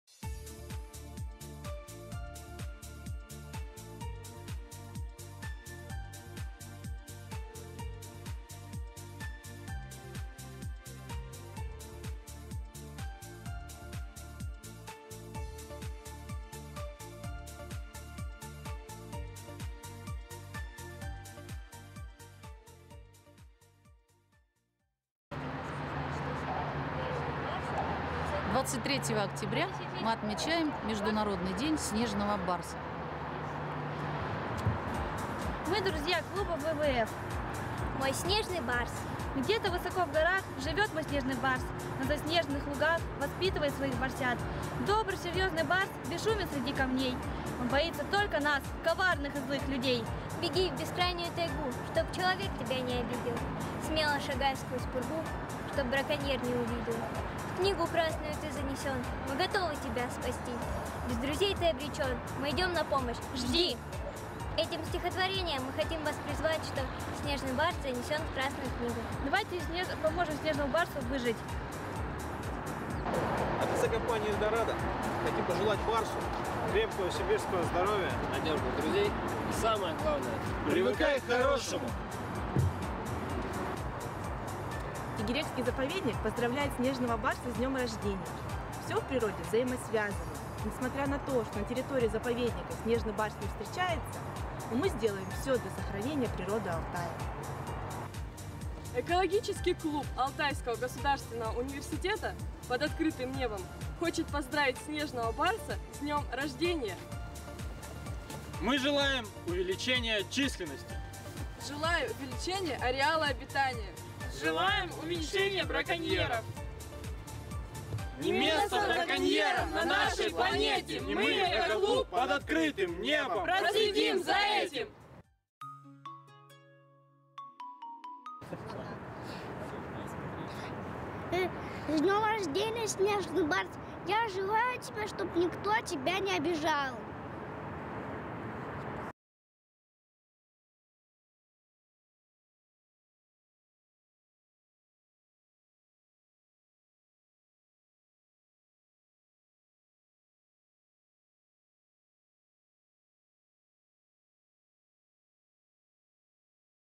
В гости к снежному барсу
Автор ролика Виктор Никулкин. Ролик сделан по заказу WWF Алтае-Саянского экорегиона. Мы принимали участие в его записи.